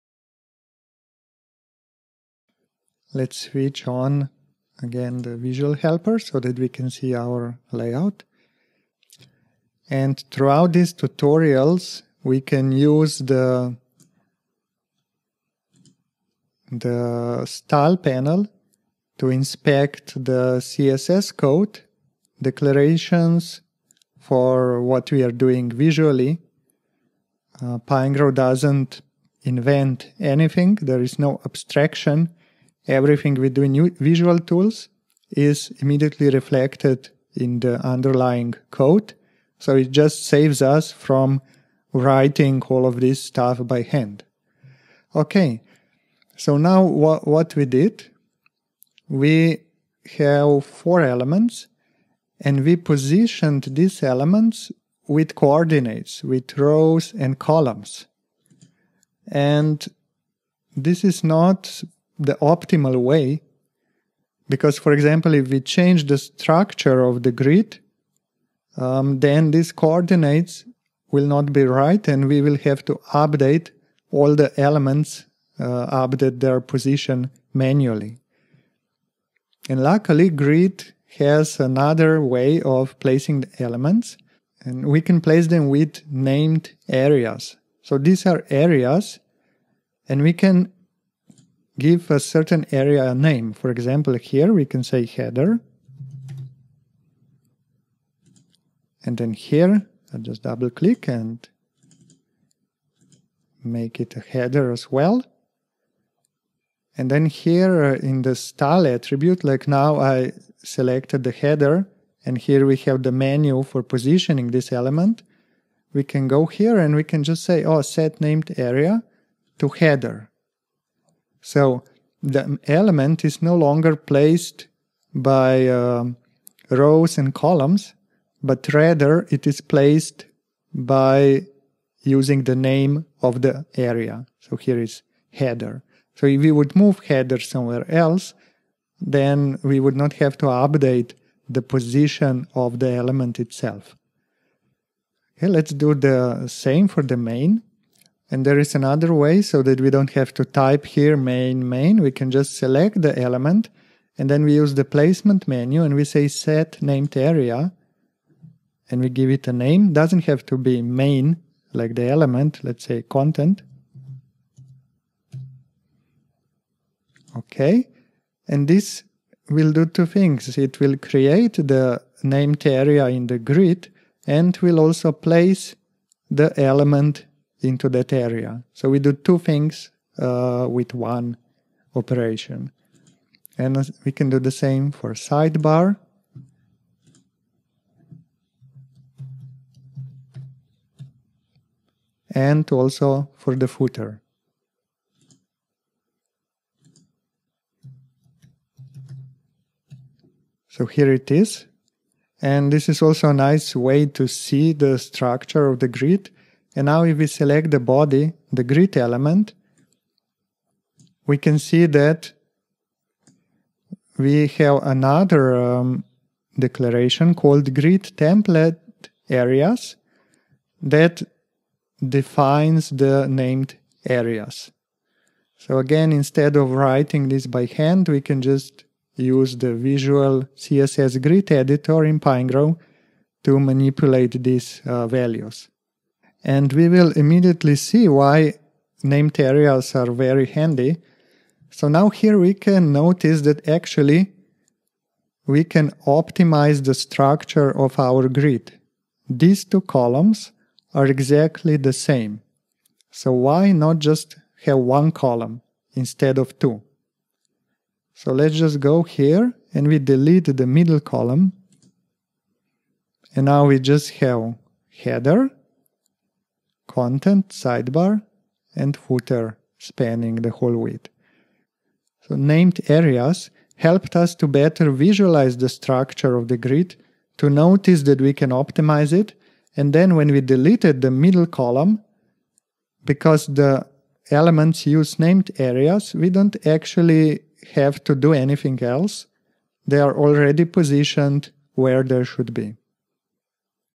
4. Positioning with named areas - Learn CSS Grid with Pinegrow
Learn how to use CSS Grid in your projects. We'll use Pinegrow Web Editor to explore and build the grid visually.